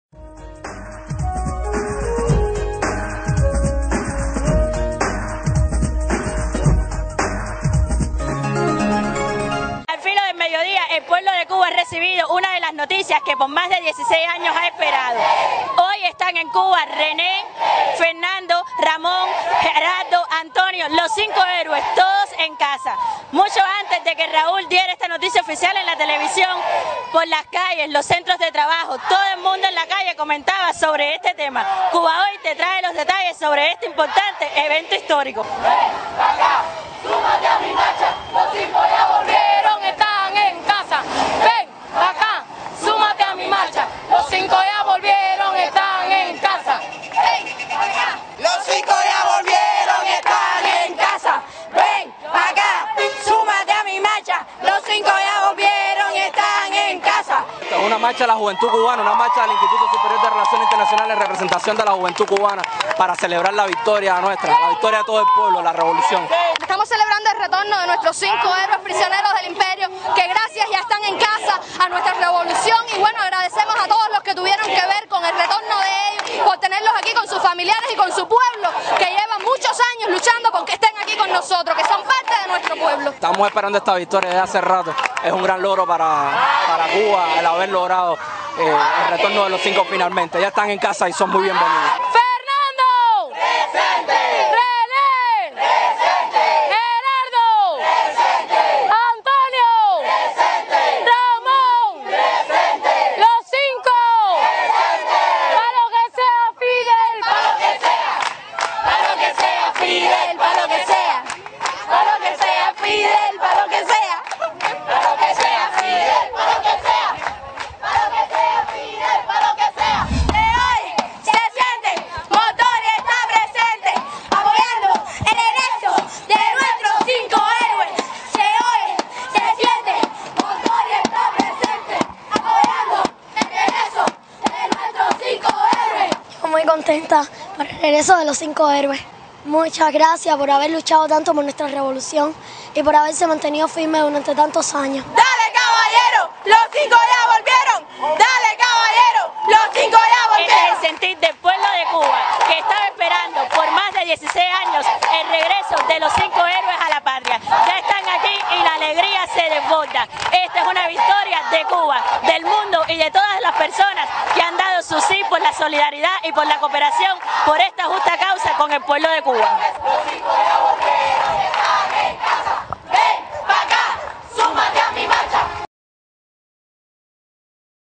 Cuba de Fiesta: Los 5 en la Patria
Estudiantes del Instituto de Relaciones Internacionales, Raúl Roa, salieron a las calles para festejar la fiesta de Cuba por el regreso de los cinco a la patria. Una noticia que hemos esperado por 16 años y que se celebra con gran regocijo.